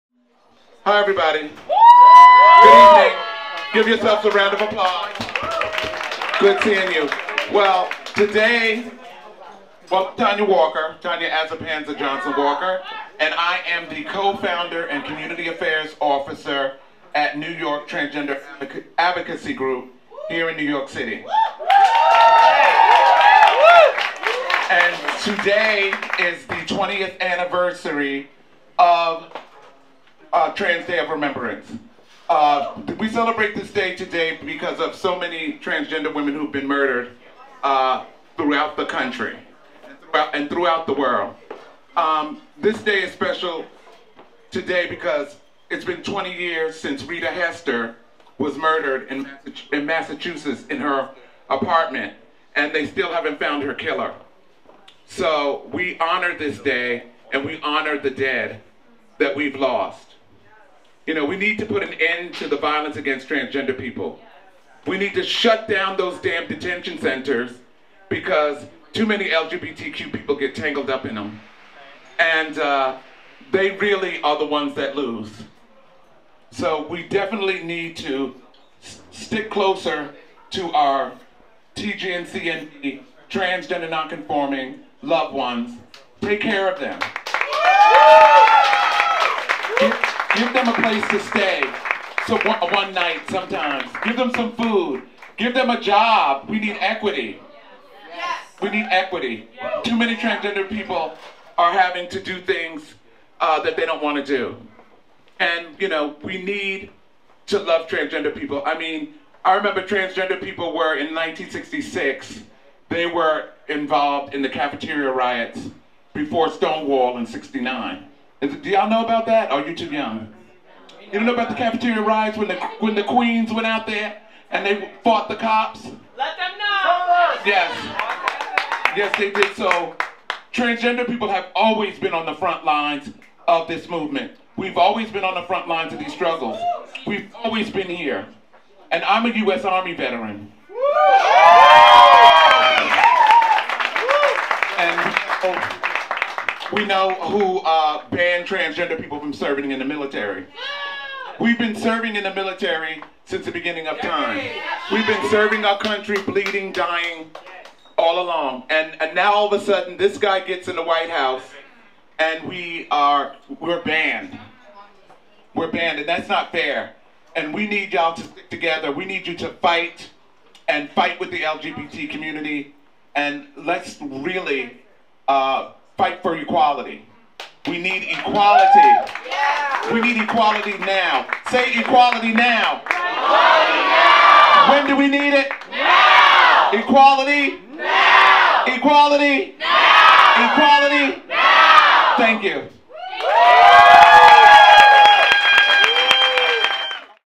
NYCTAG Speaker at Stonewall Trans Day of Remembrance
NYCTAG speaker at Manhattan's Stonewall Inn in Greenwich Villlage on the Trans Day of Remembrance in November, 2019